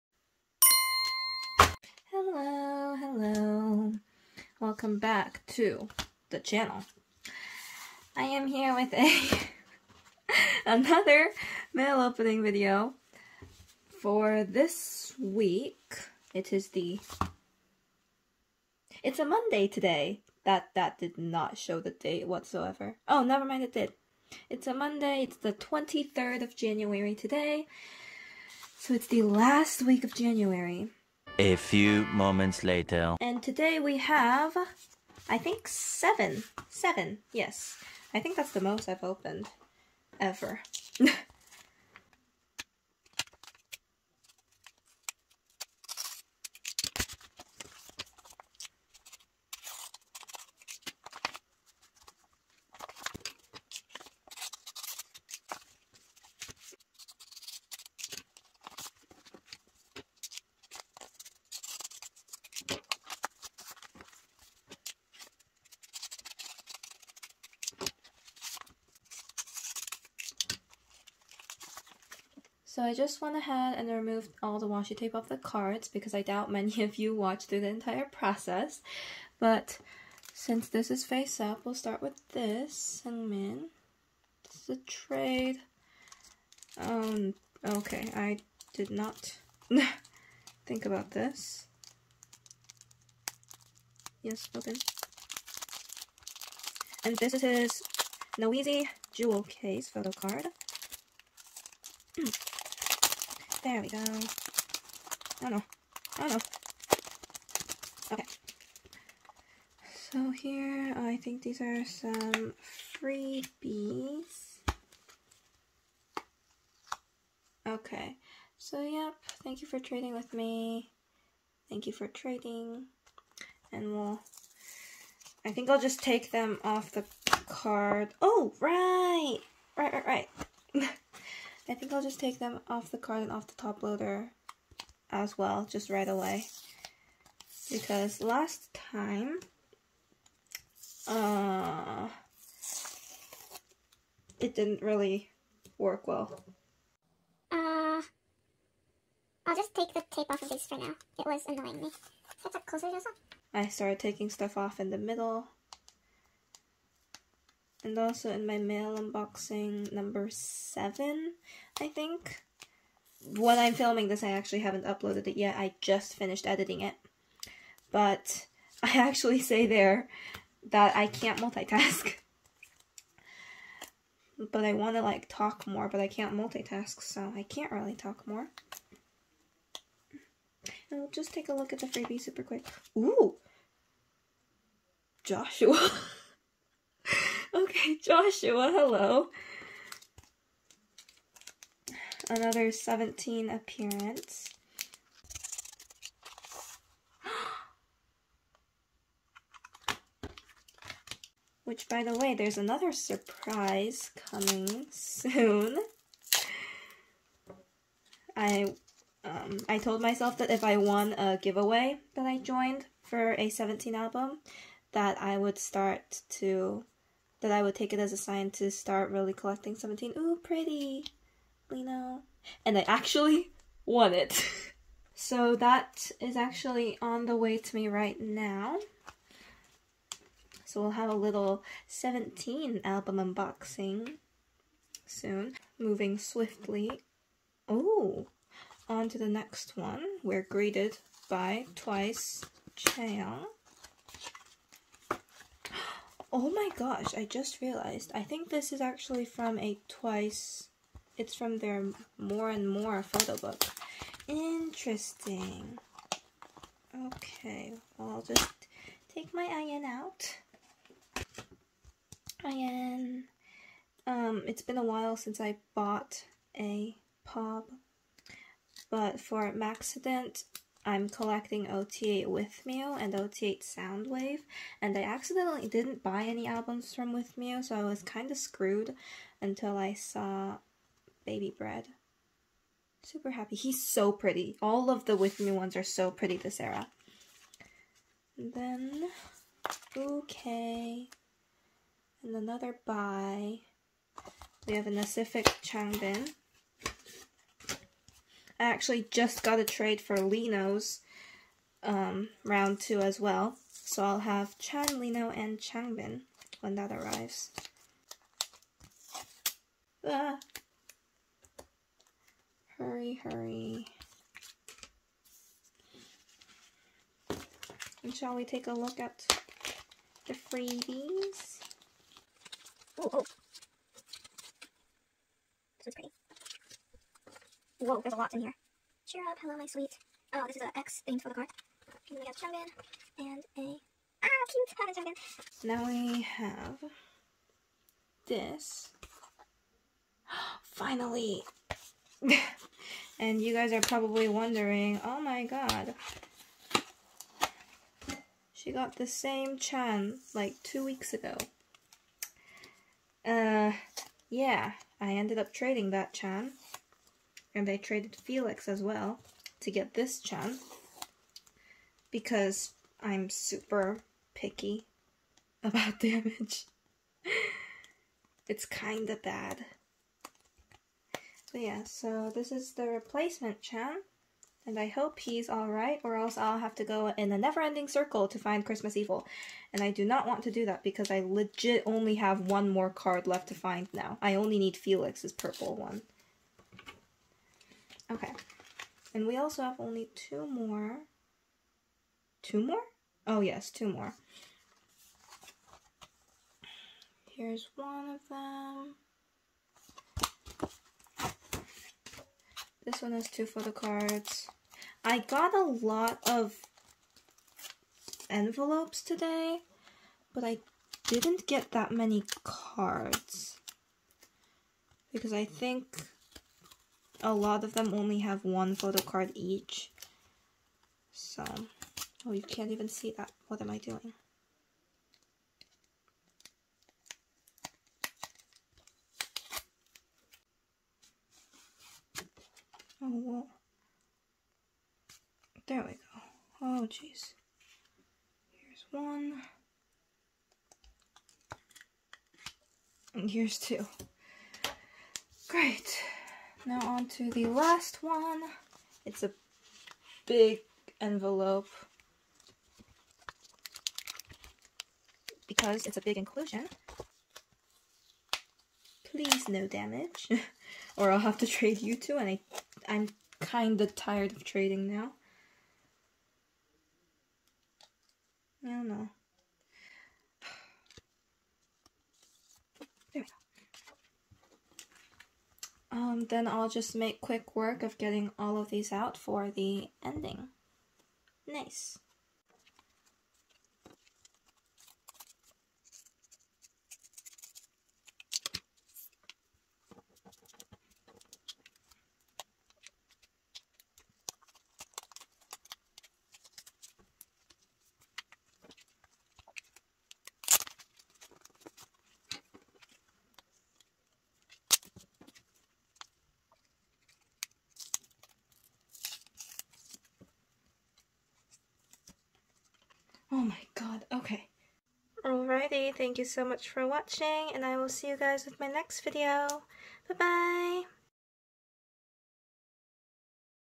[STAY VLOG #16] Stray Kids Mail Opening #8
Hi guys! Mini here☺️

So I managed to film 4 videos yesterday (1/28) (My social battery went out even when I was just talking to myself)
I'm planning to film another 3 today (1/29)
I'm not sure how many will be salvageable...😂

It's honestly insane how many videos I'll be able to crank out consistently this next month or so. I'm so excited to show you everything!

ALSO: I know I'm behind on subtitles😭 I promise I'll write the scripts and just have a day to sit down and get them done.